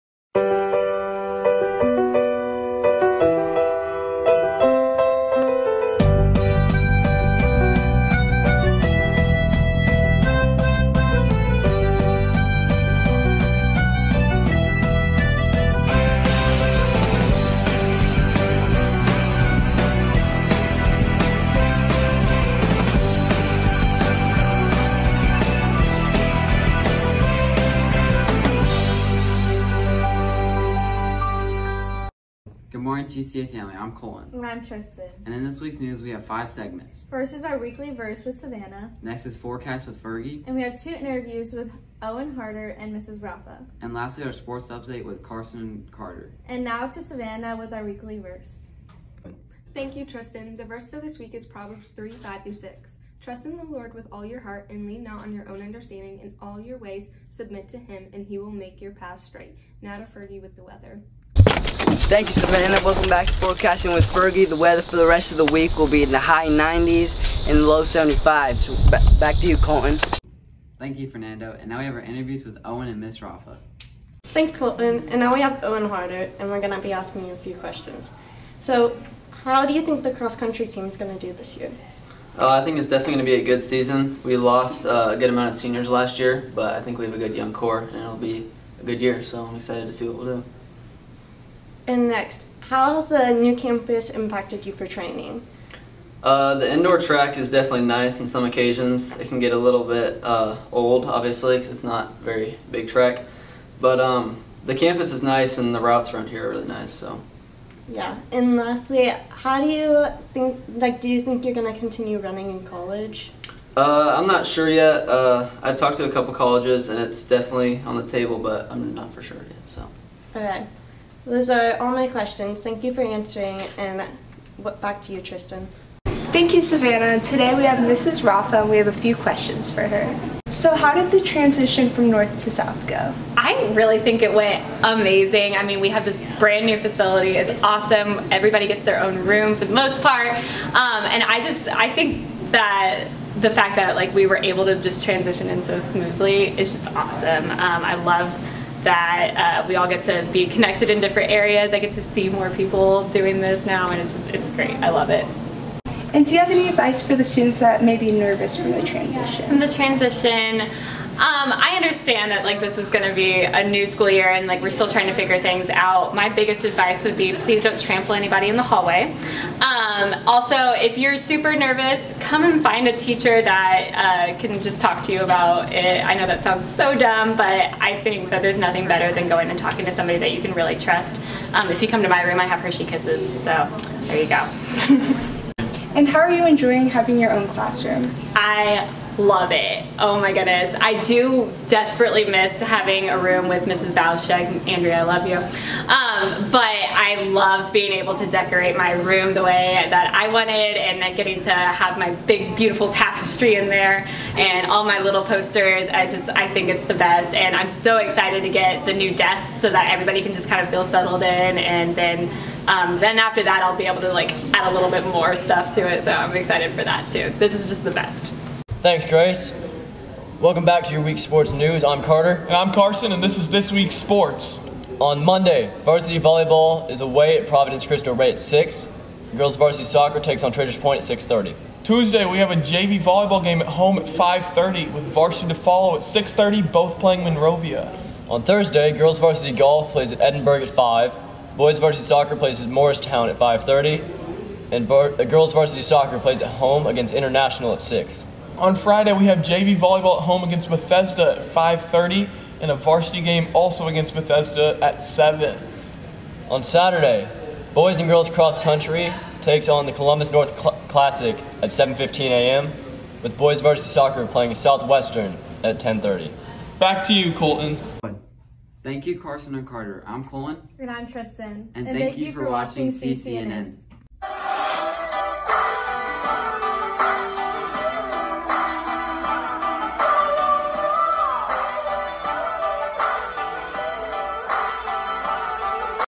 CCNN Week 2
Ferg, Carson, Carter, Grace and Emily are all back with your favorite anchors Tristyn and Colton! Hope you enjoy this weeks edition of CCNN!